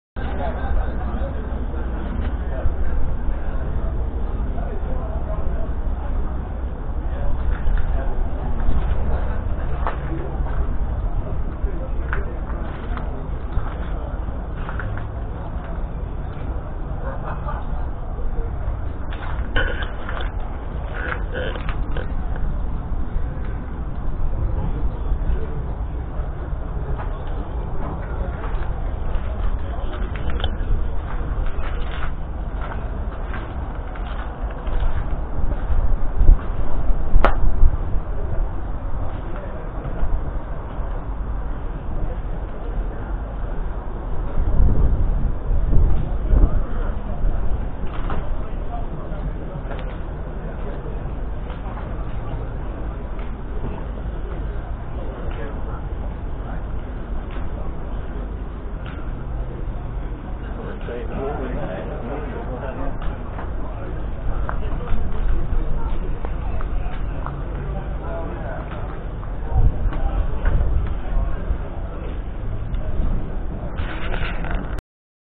2018 Claas Torion 1511 Varipower 4.5LItre 4-Cyl Diesel Articulated Wheel Loader
2018 Claas Torion 1511 Varipower 4.5LItre 4-Cyl Diesel Articulated Wheel Loader (165/167HP)

Midland Machinery Show, Newark and Nottinghamshire Agricultural Society, Newark Showground, Lincoln Road, Winthorpe, Newark-On-Trent, Nottinghamshire 21st & 22nd November 2018